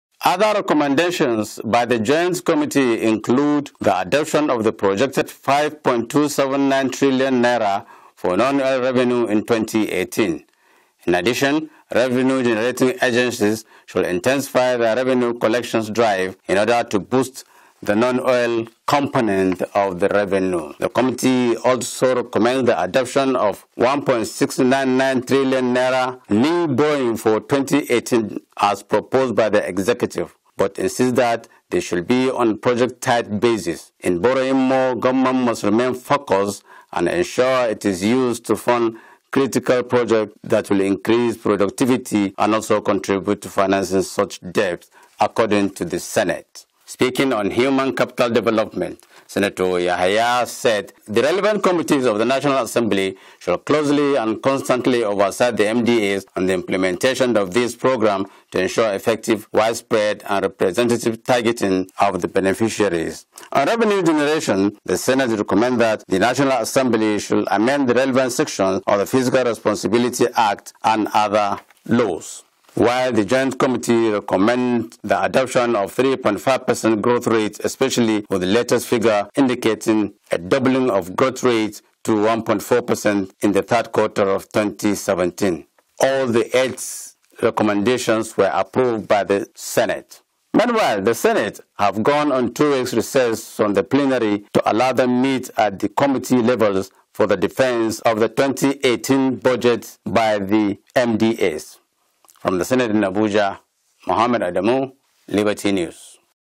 BUDGET: SENATE PASSES MEDIUM TERM EXPENDITURE FRAME WORK, RAISES OIL BENCHMARK TO $47 PER BARREL
As the Senate passed the 2018 budget proposal for the second reading, the joint committee of appropriation, finance and planning has submitted 8 recommendations in the 2018 medium term expenditure framework at the plenary before adjourning of sitting.
A member of the committee, Abdullahi Yahaya said that the committee recommended the oil benchmark be raised from 45 dollars per barrel to 46 dollars. However, the Senate at plenary adopted 47 dollars per barrel. It also adopted exchange rate of 305 naira to a dollar for 2018 fiscal year.